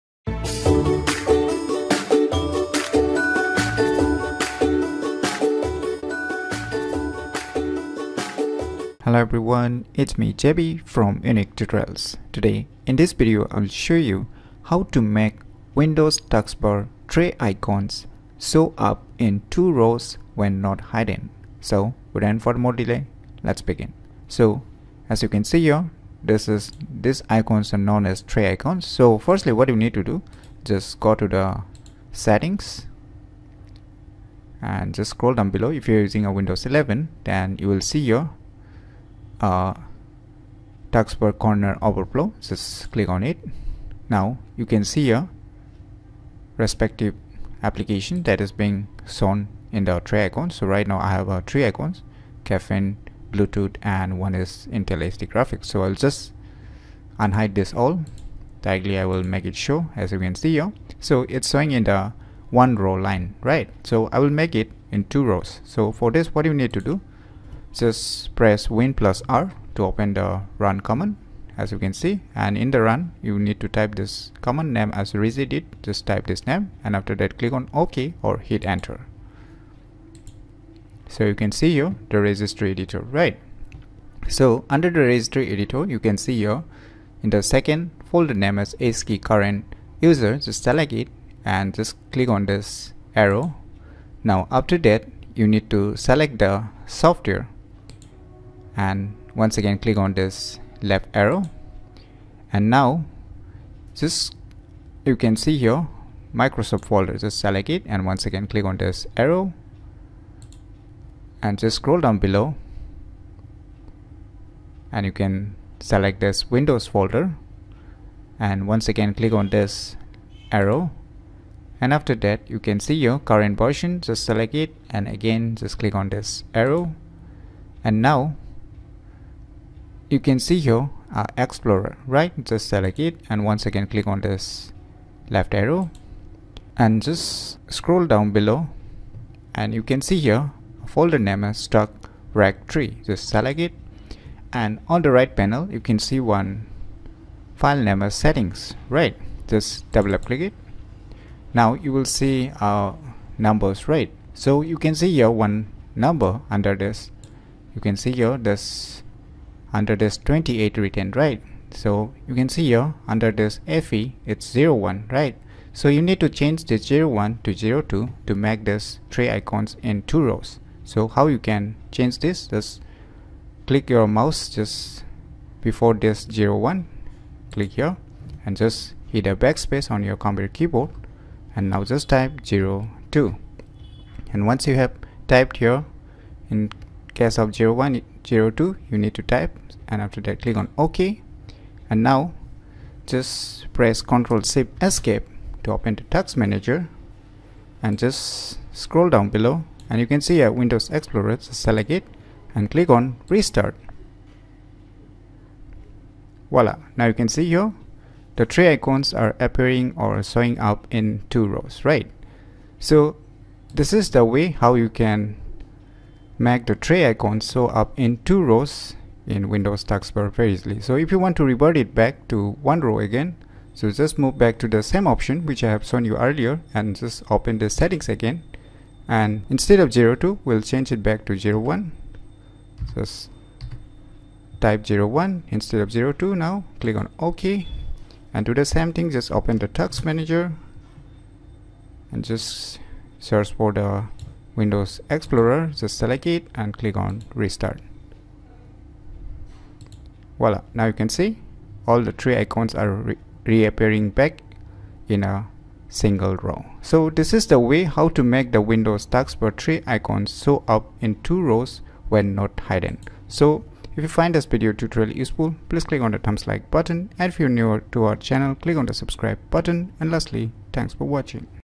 How to make Windows Taskbar Tray Icons show up in two Rows when not Hidden ?
This video tutorial is all about How to make Windows Taskbar Tray Icons show up in two Rows when not Hidden.